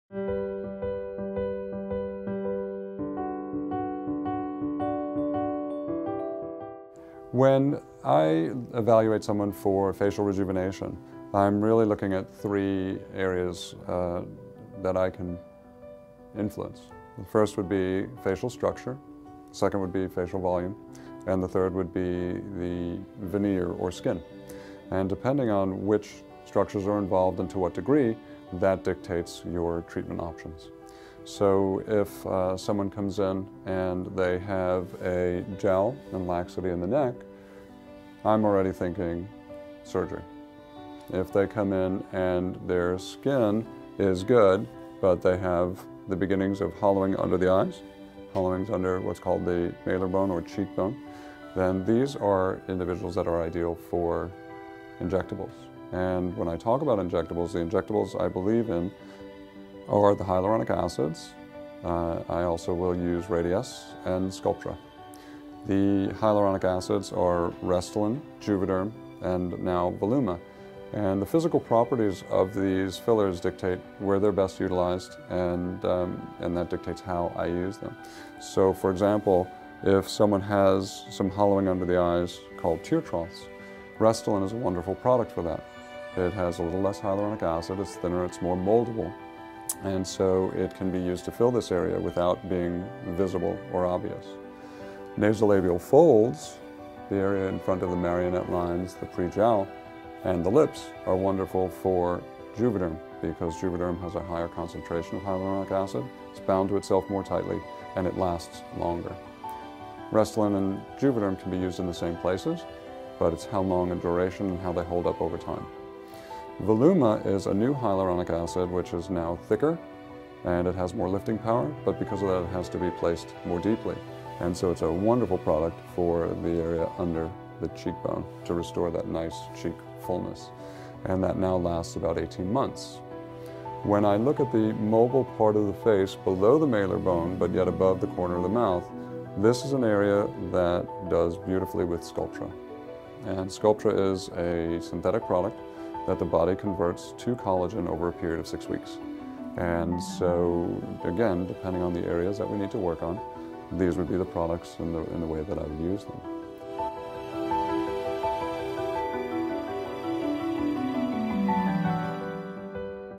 Facial Fillers and Injectables for Facial Rejuvenation Explained by Dr. Rob Kessler
Are injectables and fillers right for you? Dr. Kessler explains the differences of each type and who they are best for.